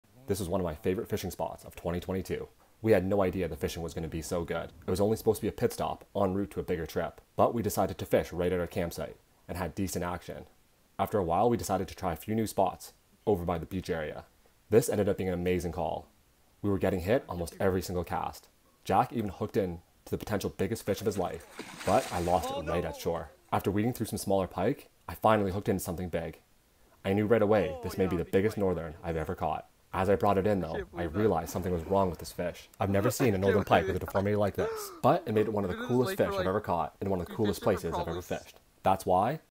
I found the best fishing spot (by accident)
We were enroute to our fly in fishing trip we decided to camp out at this provincial park... little did we know that it would such an amazing spot, we will definitely be going back here in 2023 to spend some really time on the lake.

Counting down my top 8 fishing moments from 2022. It was an amazing year full of first from musky to steelhead to flying on my first bush plane. It was a great year and I am really grateful for all the support from you guys! So thank you and hope you guys get to spend lots of time on the lake in 2023!

 @Time on the Lake